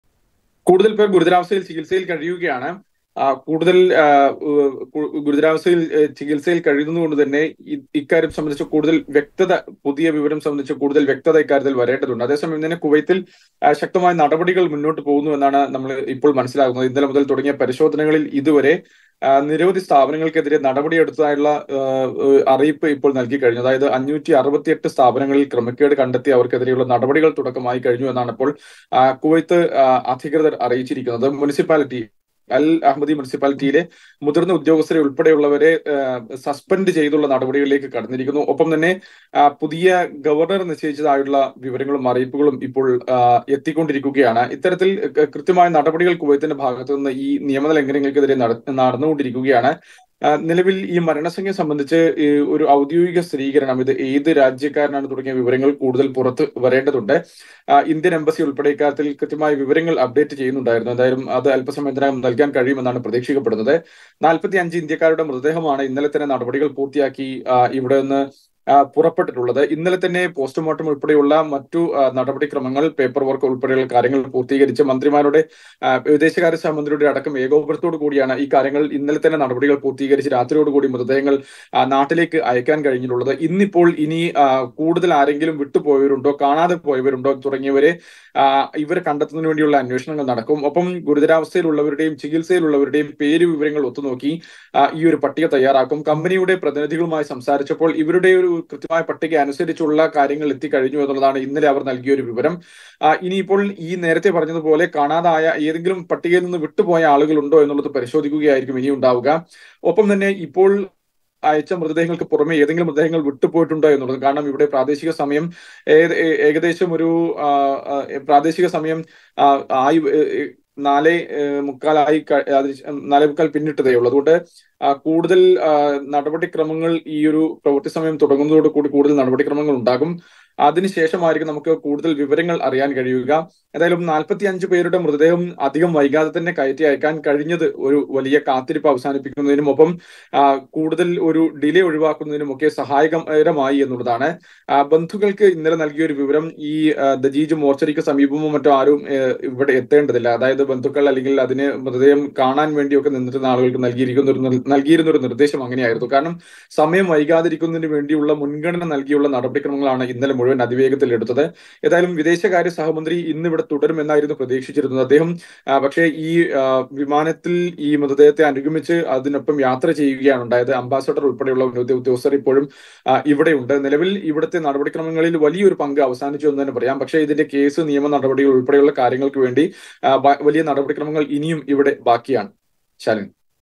കുവൈത്ത് ദുരന്തം : മരണം 49 ആയി
കുവൈത്ത് ദുരന്തം : മരണം 49 ആയി, ആശുപത്രിയിൽ ചികിത്സയിൽ ആയിരുന്ന ഒരു ഇന്ത്യക്കാരൻ കൂടി മരിച്ചതായി കുവൈത്ത് മാധ്യമങ്ങൾ